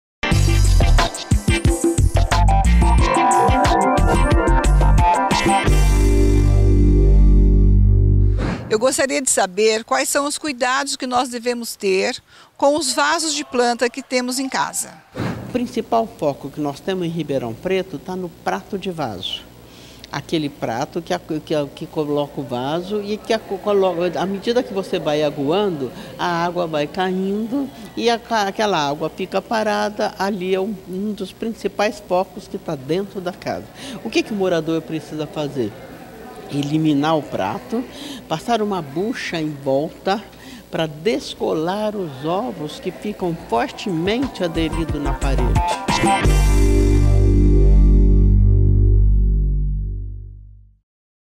Unaerp Mobiliza - Cuidados com o vaso de planta para evitar a procriação do mosquito
Vídeos sobre a transmissão dos vírus da Dengue, Zika e Chikungunya tem como objetivo levar informações e conscientizar a população. Campanha #‎unaerpmobiliza